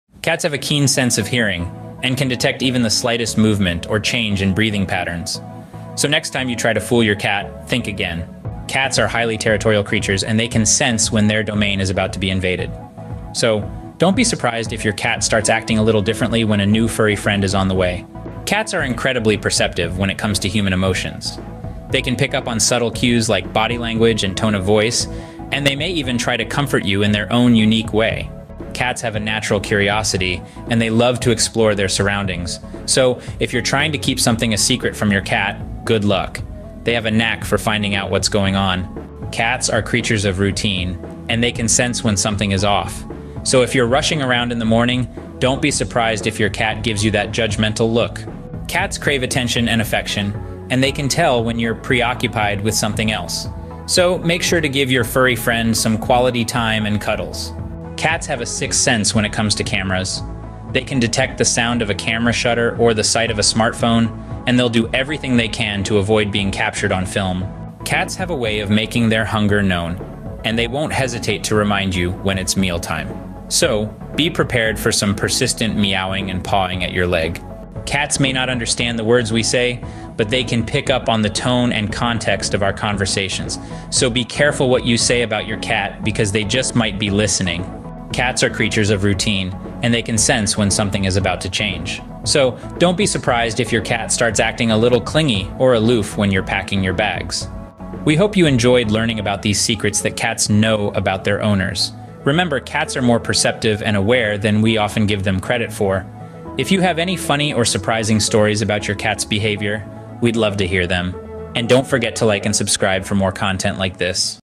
10 Things Your Cat Knows About You That You Don't!
Discover the secrets your furry friend is keeping with our latest YouTube Short, '10 Things Your Cat Knows About You That You Don't!' This video peels back the curtain on the mysterious world of our feline companions, revealing how much they truly understand about us. From their uncanny ability to detect your moods to knowing when you need that extra bit of comfort, cats are more in tune with us than we might think. Whether you're a devoted cat owner or just a lover of all things feline, this video will give you a humorous and insightful look into the secret life of cats. Don't miss out on uncovering the ten surprising things your cat knows about you. Perfect for cat enthusiasts and animal lovers alike, this video combines scientific insights with a dash of humor, making it a must-watch.